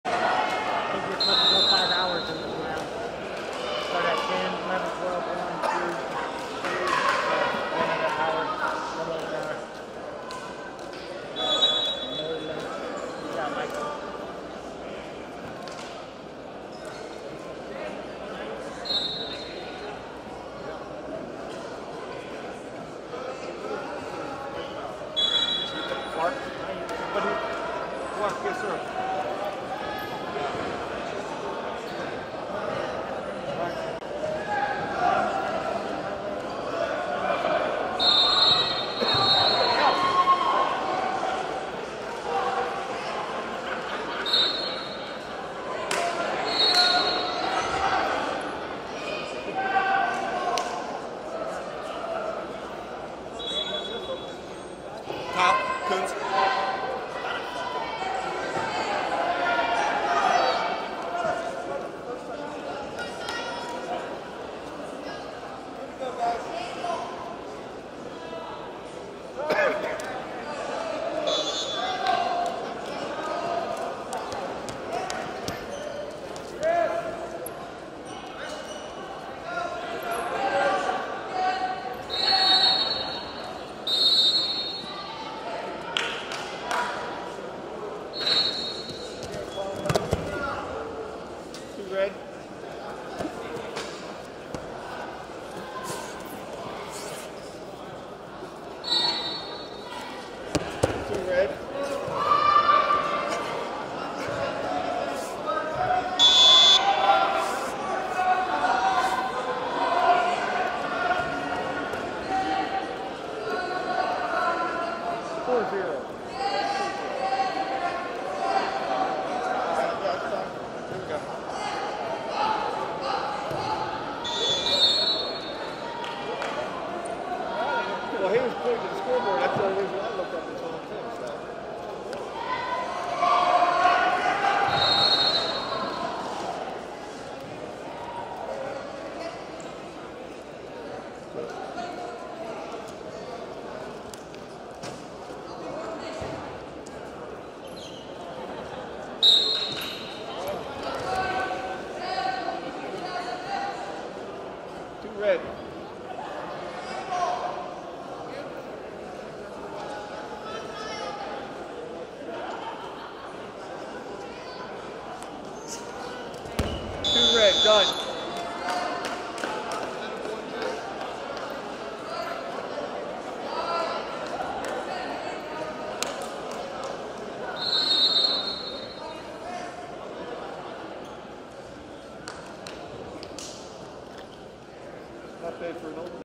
97 Kg Quarterfinal - Jacob Clark, Minnesota Storm Vs Kyle Kunz, Arizona
FloSports/2021 Last Chance US Olympic Trials Qualifier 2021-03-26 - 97 Kg Quarterfinal - Jacob Clark, Minnesota Storm Vs Kyle Kunz, Arizona